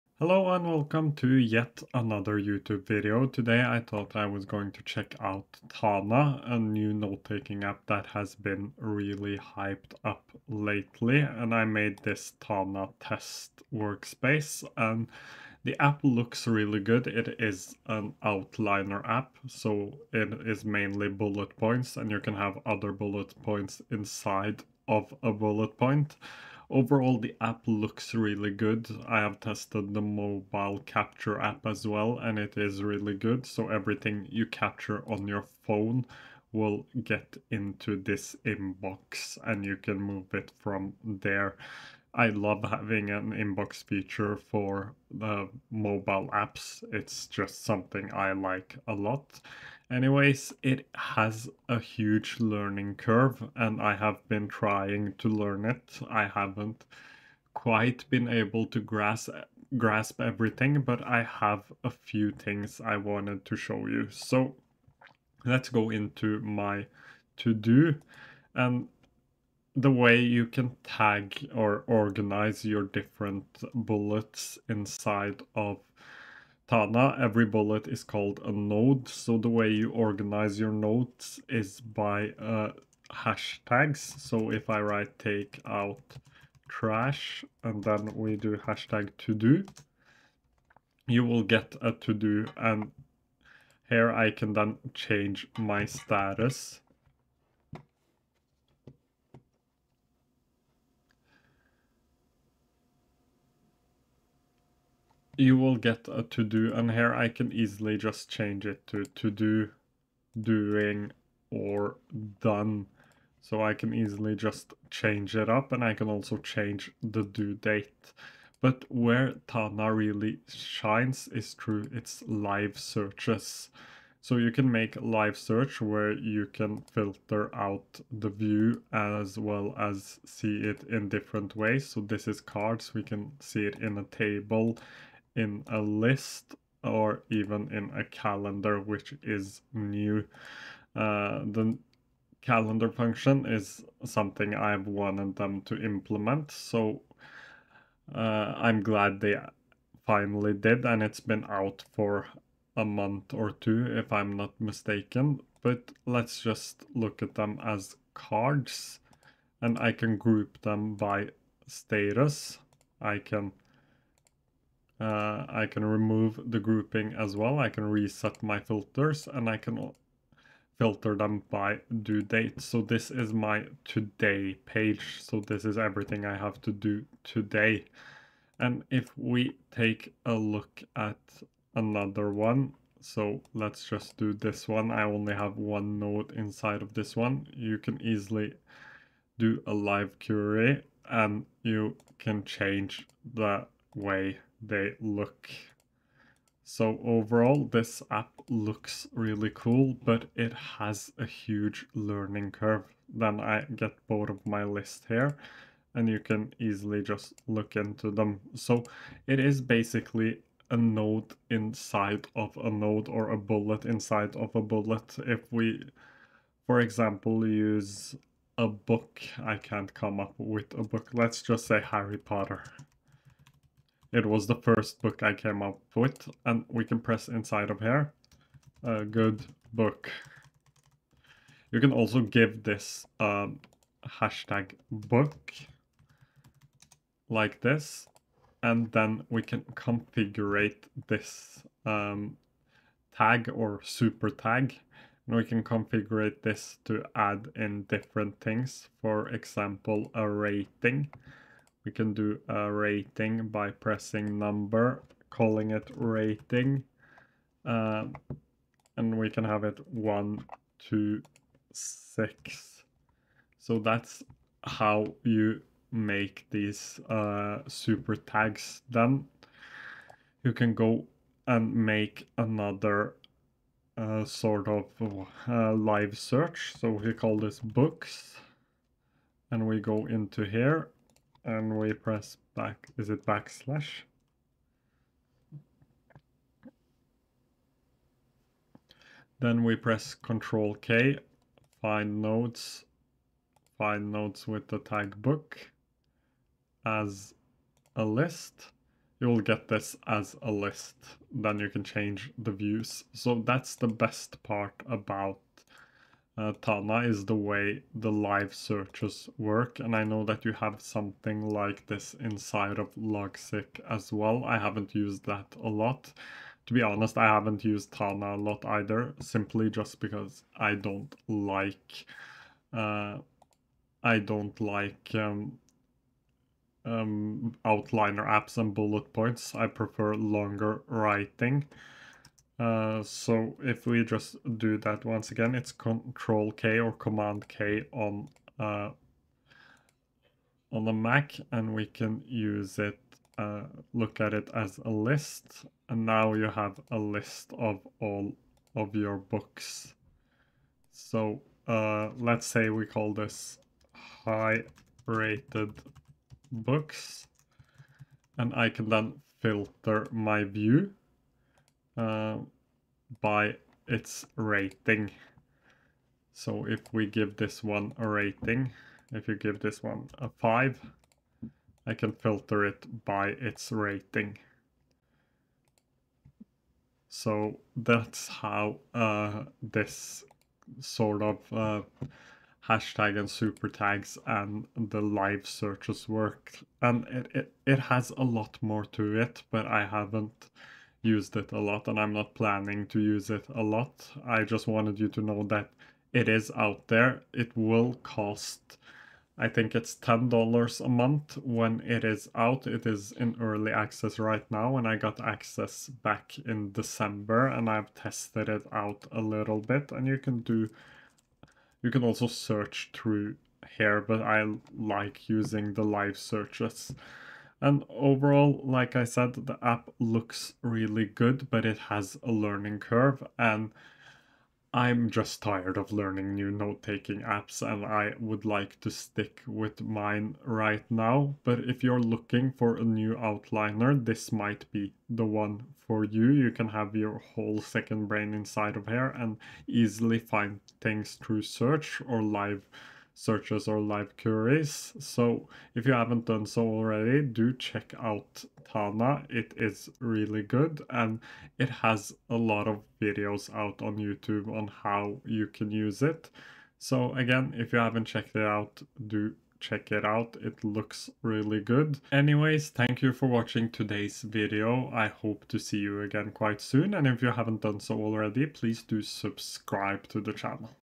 Tana a new note taking app
Tana is a new note taking app and pkm tool that has gained a lot of traction. Tana is an outliner app like roam or logseq, but is it better?